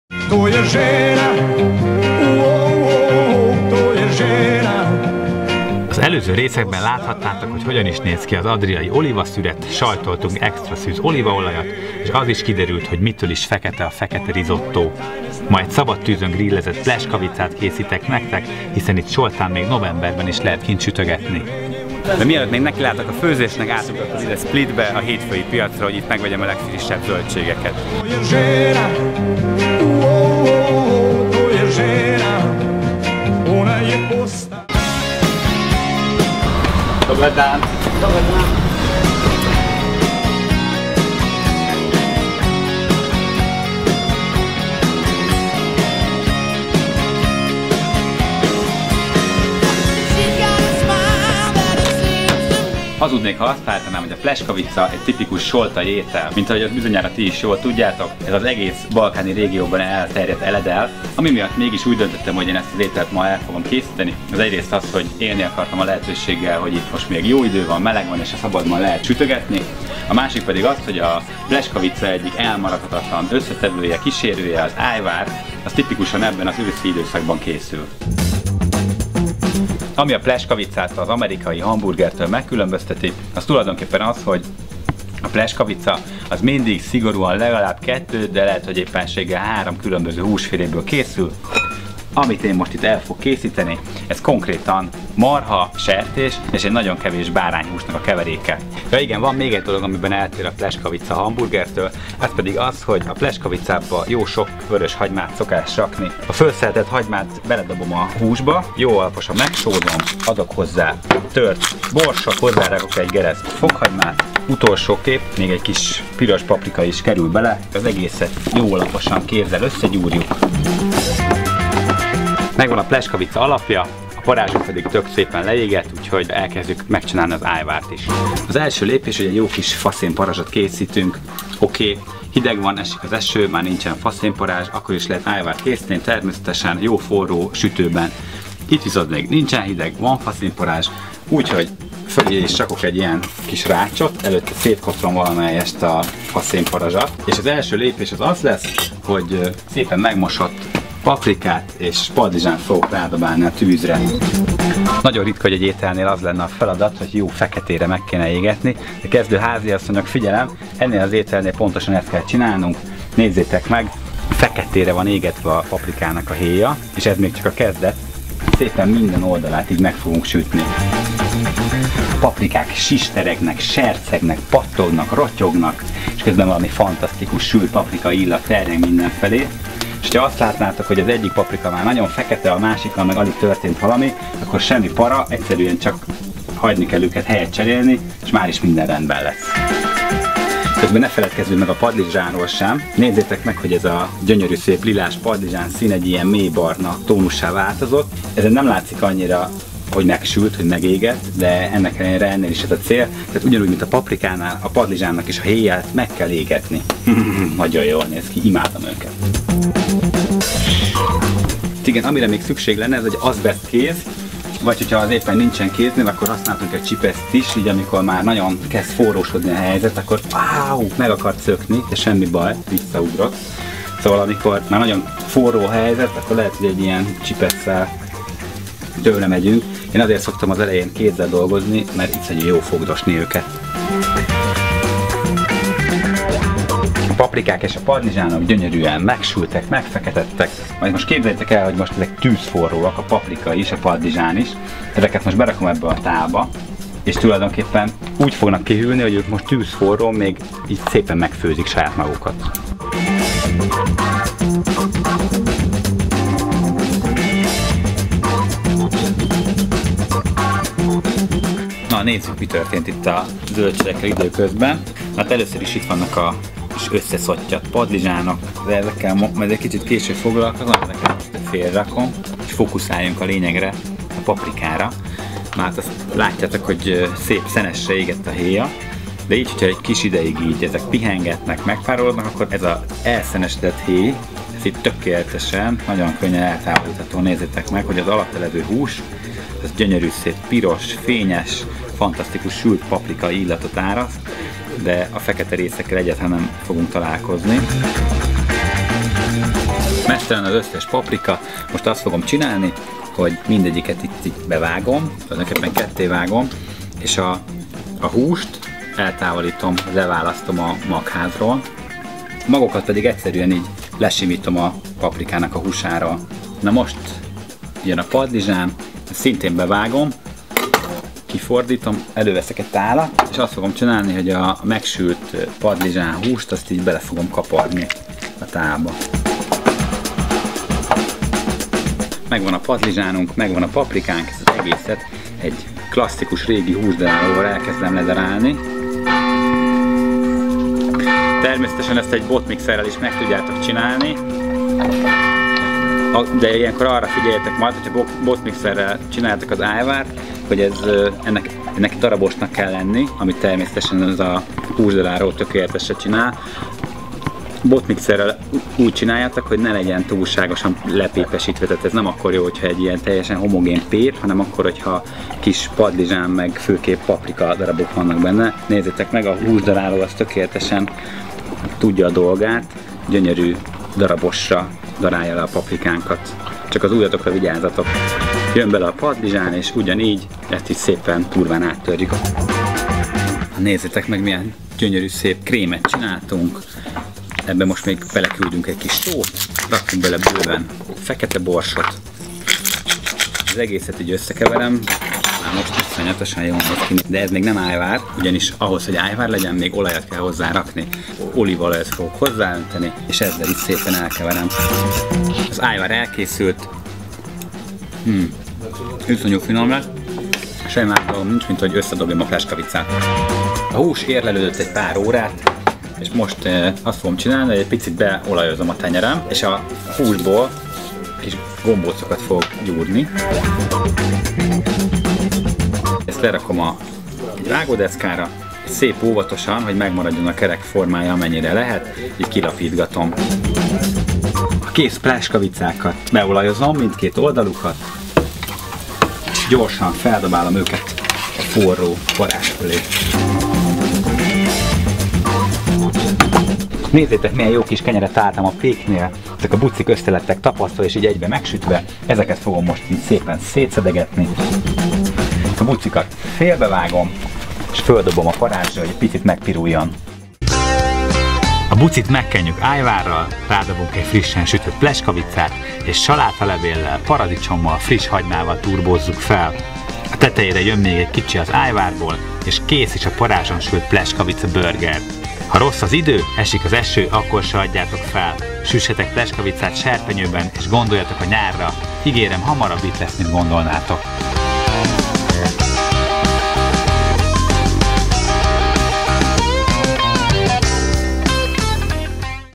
Őszi gasztrotúra az Adrián -- III. rész: Pljeskavica + ajvár
A horvátországi olíva-trilógia befejező része, melyben bebizonyítom, hogy ősszel is lehet grillezni! Recept a blogon

Nézzétek meg az első két részt is!